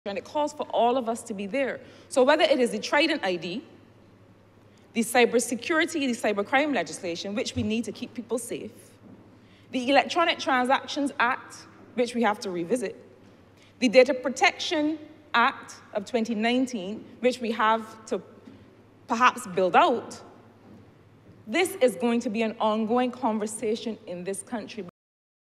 Data Centre to power local data and tech industry
Minister of Industry, Innovation, Science and Technology Marsha Caddle is confident that her Ministry is poised to lead Barbados on the path to economic growth.

Minister Caddle says the proposed Data Centre outlined in the Budget is going to power the promising data and technology industry.

She says this is set to create opportunities for many Barbadians.